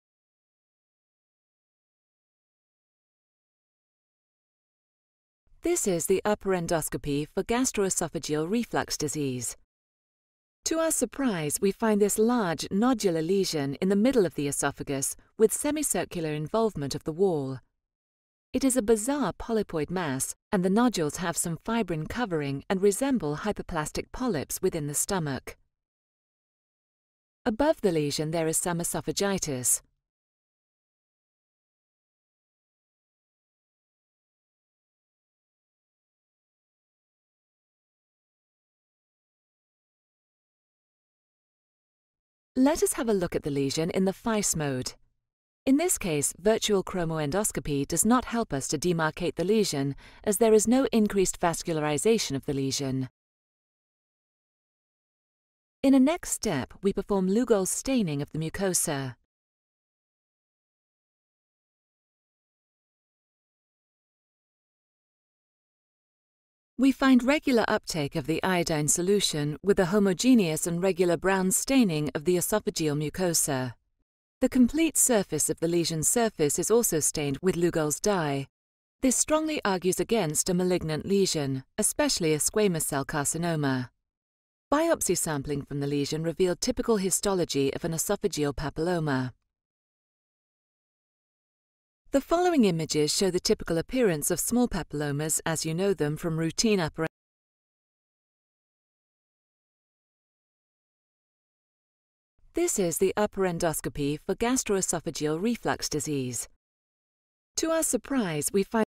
Endoscopic Imaging of a Large Esophageal Papilloma
Abstract: Esophageal papilloma is a rare endoscopic finding. Lesions are usually less than 5 mm, appear as a solitary sessile nodule, and are located in the middle or upper part of the esophagus. The surface has a warty-like structure. Malignant potential of esophageal papilloma is unknown. The essential differential diagnosis of esophageal papilloma comprises squamous cell, verrucous carcinoma, and hyperkeratotic lesions. Owing to missing data there are no therapeutical recommendations. In a pragmatic approach, lesions should be removed endoscopically. This article is part of an expert video encyclopedia.